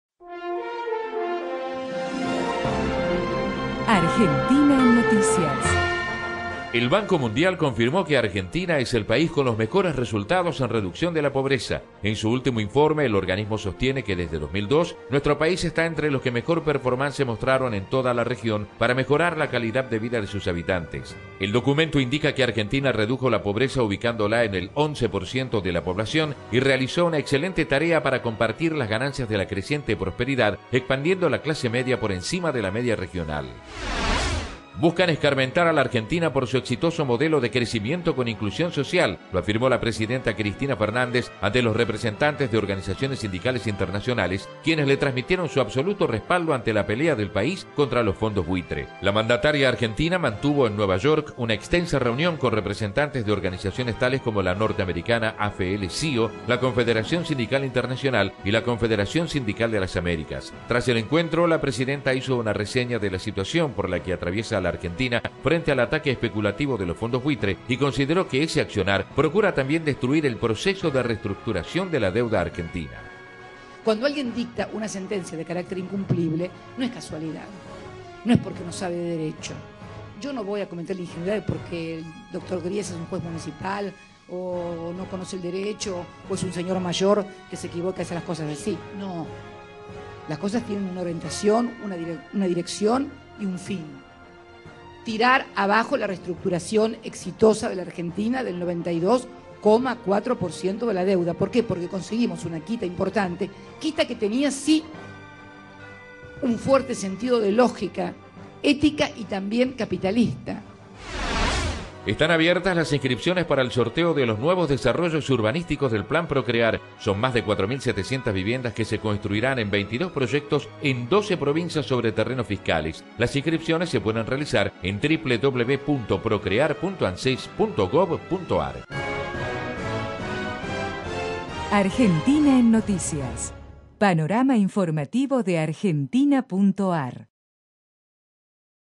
Inscripción para Pro.Cre.Ar - Apoyo a CFK de organizaciones sindicales. AEN 23-09 11HS.
El banco mundial confirmó que Argentina es el país con los mejores resultados en reducción de la pobreza.

"Buscan escarmentar a la Argentina por su exitoso modelo de crecimiento con inclusión social", lo afirmó la presidenta Cristina Fernández ante los representantes de organizaciones sindicales internacionales.

Están abiertas las inscripciones para el sorteo de los nuevos desarrollos urbanísticos del Plan PRO.CRE.AR.